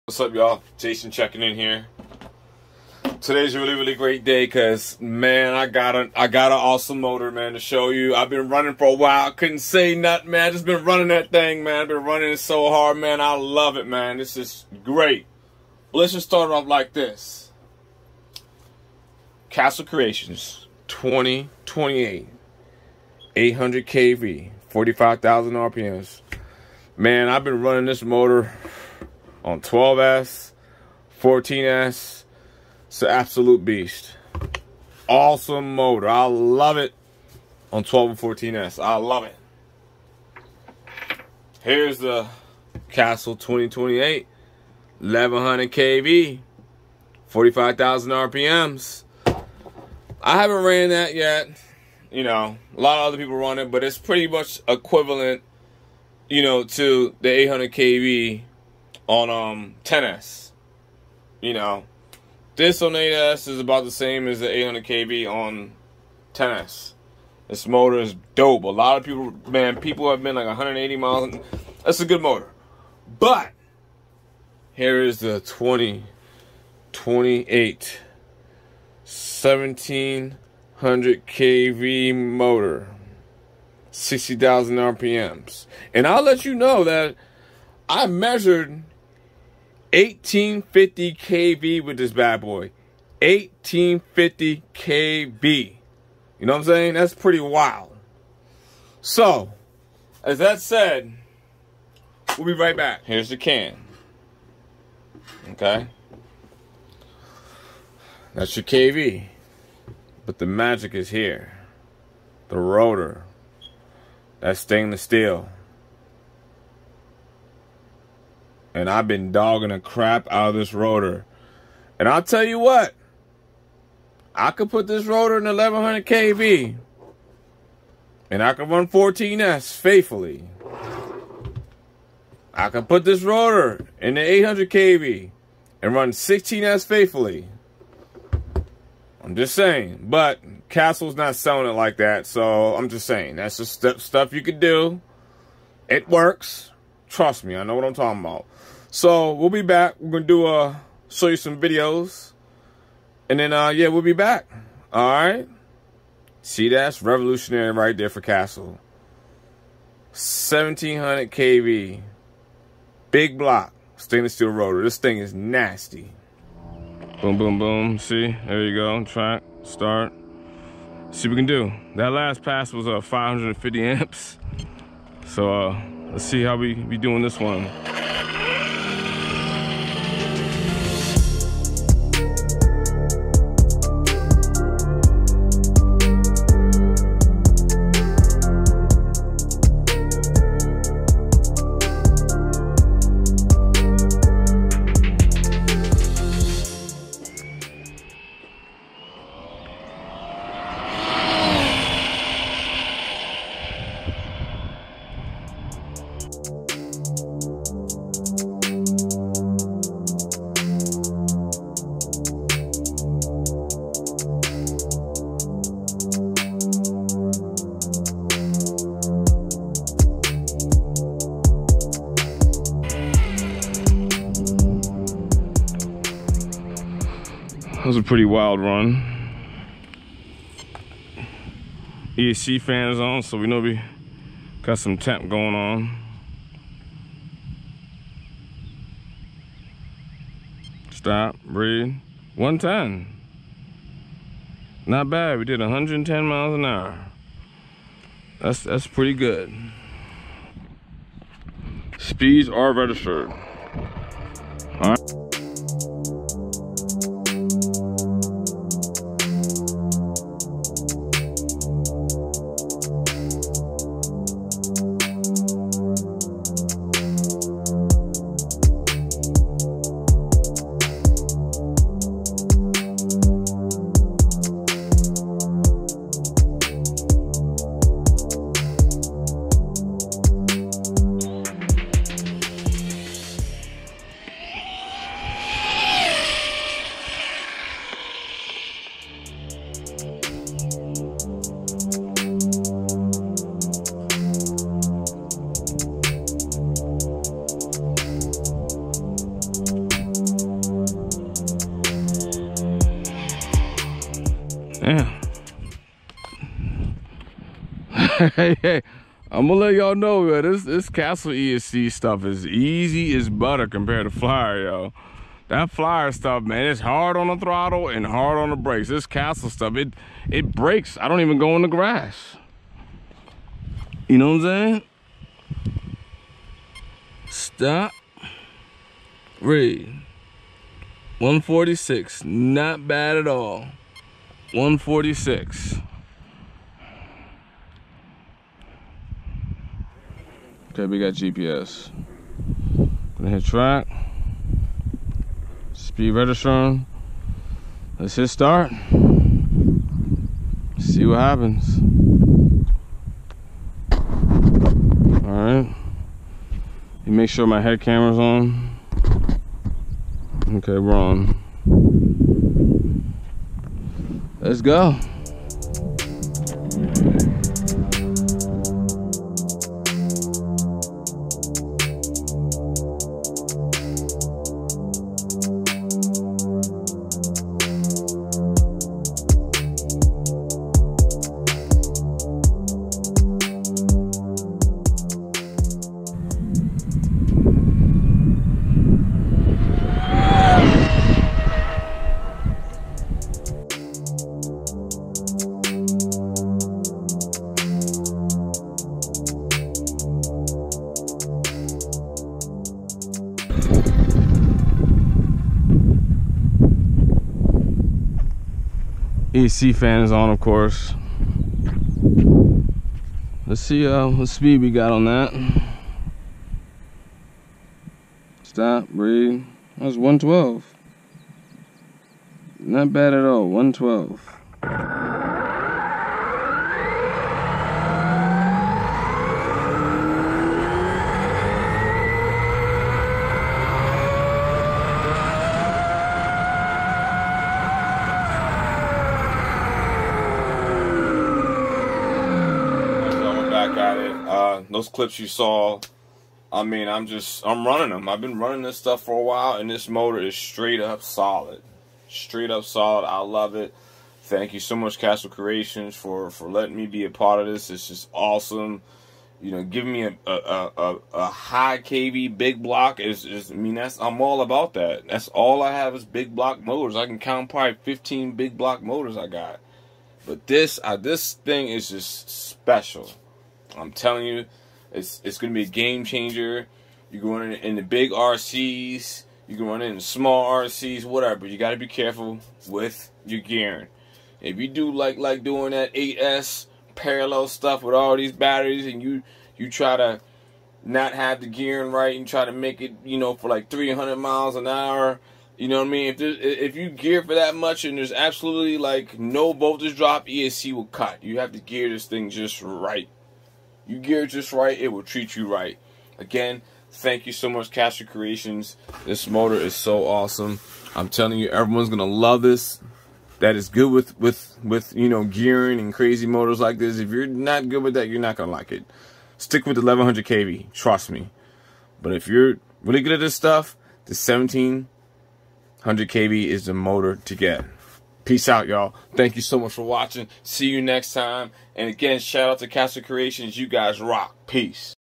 [NEW]Castle Creations 2028 1700kv🔥🔥🔥🔥😱
Finally the day is here. Man I tell you this motor is the key to big scale speed running, not to mention it will dominate 1/7 platforms with 2500 feet of room to run.  Gearing is key with this motor, start off small, make your run, check data and move up slowly.  I had a lot of fun testing this motor for Castle Creations.  I hope everyone enjoys the video.   Thank you Castle  Creations for letting me test out this motor.